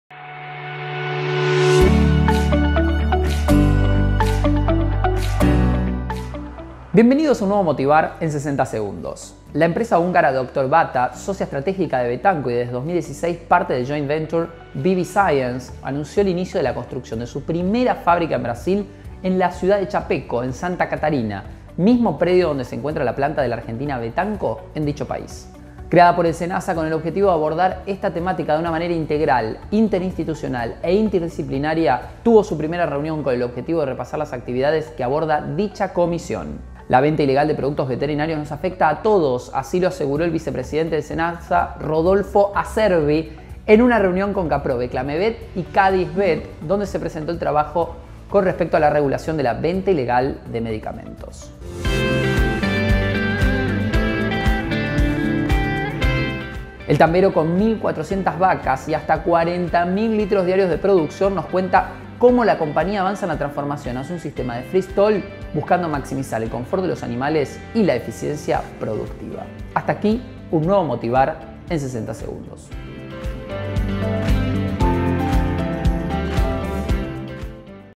MOTIVAR en 60" | Nueva edición, más contenido
En esta nueva edición de MOTIVAR en 60 segundos, Facundo Sonatti nos resume las principales novedades de la Industria de la Sanidad Animal.